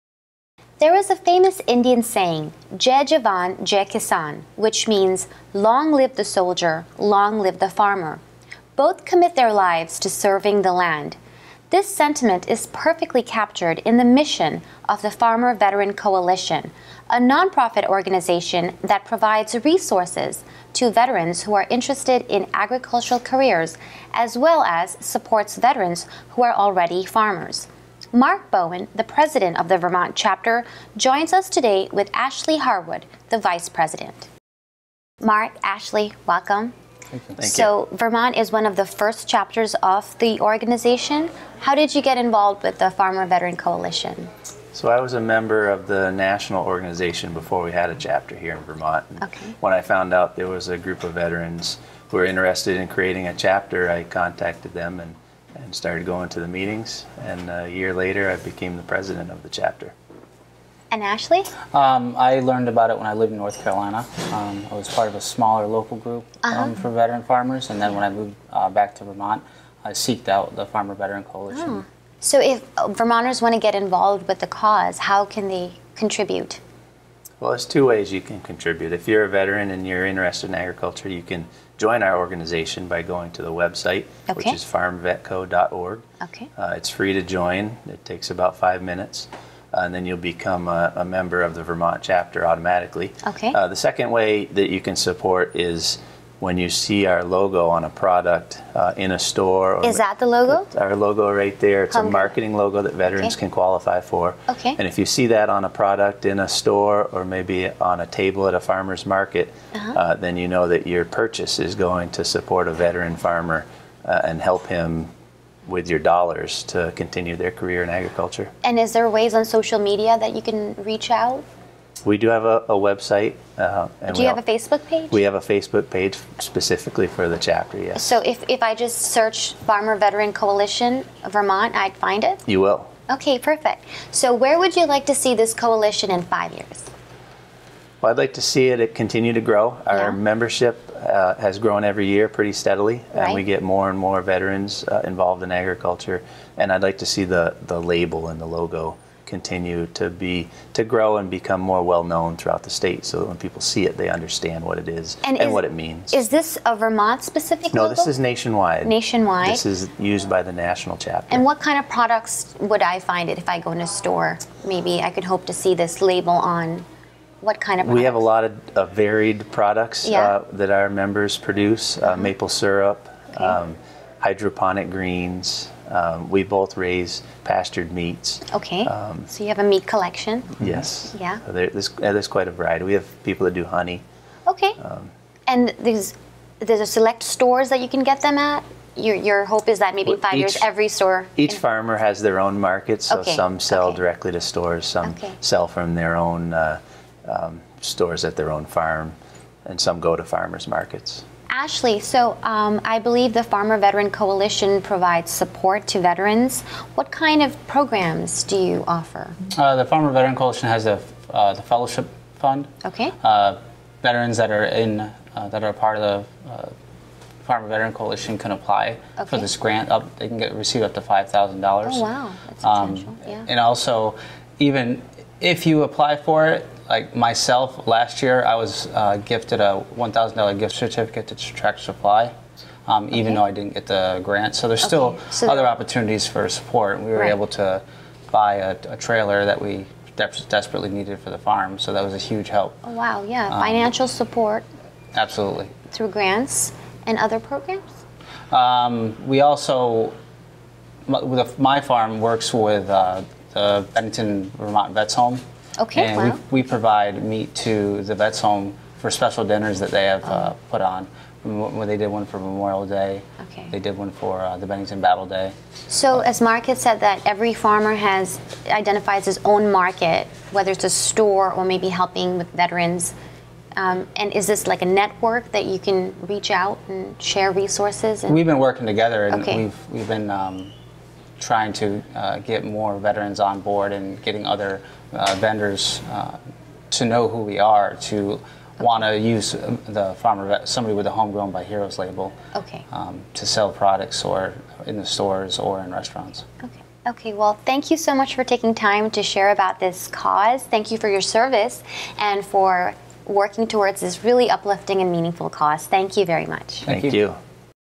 Dialogues with Meg Hansen Featuring Mark Bowen & Ashley Harwood of the Farmer Veteran Coalition
This week Meg dialogues with Mark Bowen, and Ashley Harwood, president and Vice President of the Vermont chapter of the Farmer Veteran Coalition. They discuss how the organization assists veterans of the armed forces in the pursuit of a career in agriculture. From marketing considerations in the form of their seal on products, to grants for purchasing necessary equipment to grow and expand veteran owned farms.